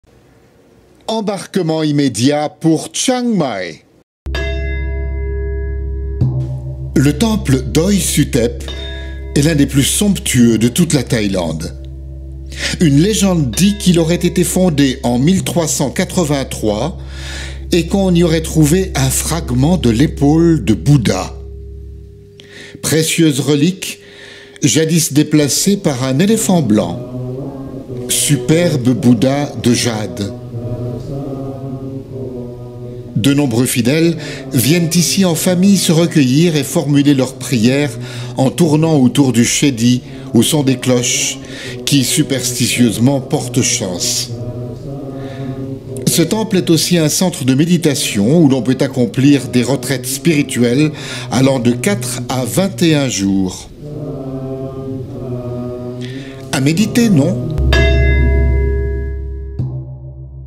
Wat Phrathat Doi Suthep, temple de Thaïlande
Un voyage en Thaïlande ne devrait jamais faire l'impasse sur Chiang Mai, au nord du pays. On y fait la visite de temples superbes pour leur architecture et ce qu'ils contiennent. On y trouve des moines (parfois même en méditation) et de belles statues de Bouddha en jade.
pour en savoir plus !